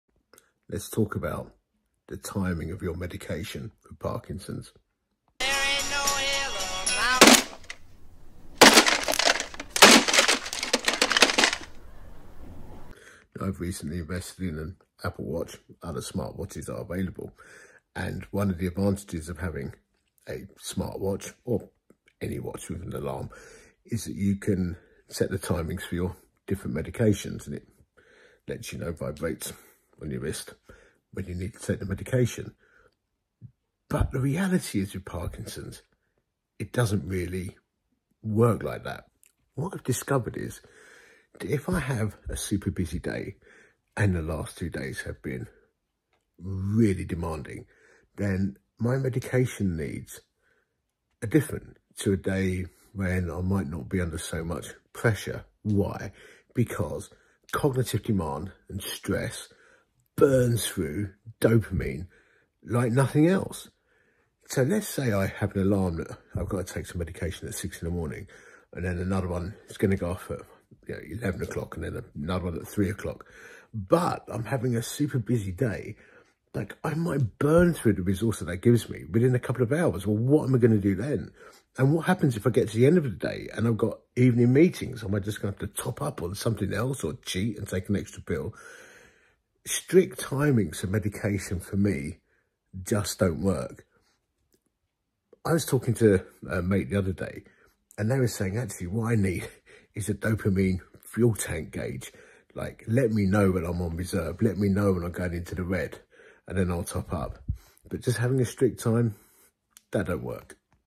Do you need to take Parkinson’s medication on time?
I’m not convinced that levodopa is time critical.